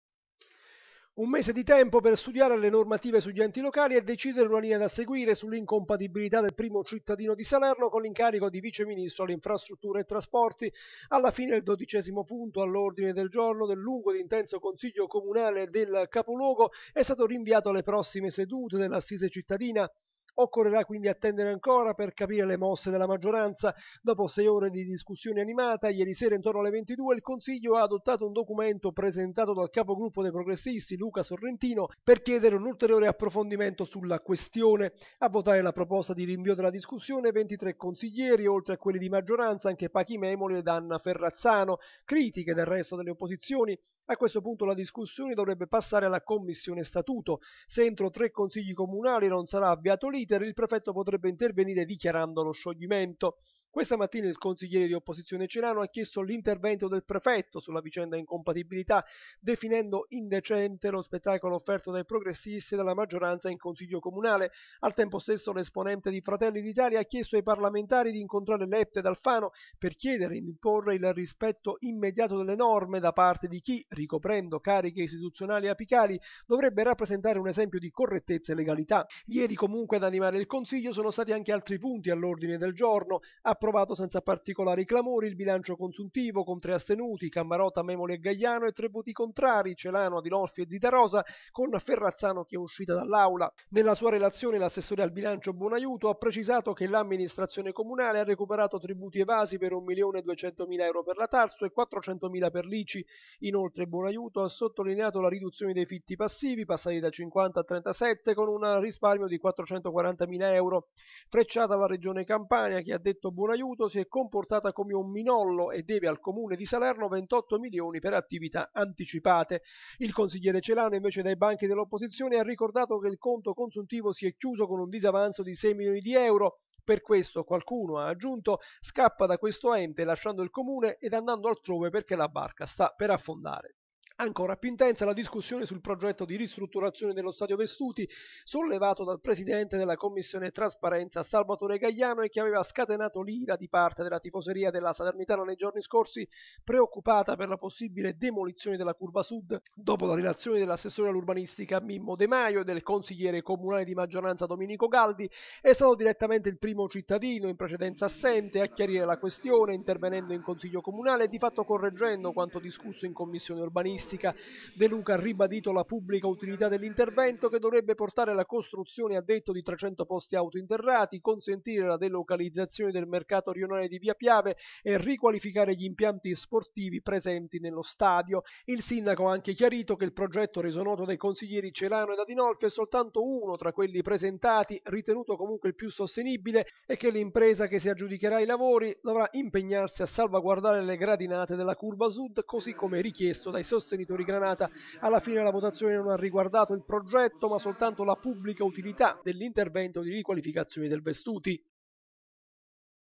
Incompatibilità, se ne riparla tra un mese. Il consiglio comunale di Salerno si esprime sul Vestuti
Oltre sei ore di discussione, ieri per il consiglio comunale di Salerno, terminato a notte inoltrata. Slitta di un mese la discussione sulla incompatibilità del sindaco De Luca dopo la nomina a vice ministro. Approvato il bilancio consuntivo ed anche la ristrutturazione dello stadio Vestuti, seppur con alcuni correttivi e la promessa di salvaguardare la curva sud. (010613 Giancarlo Frasca)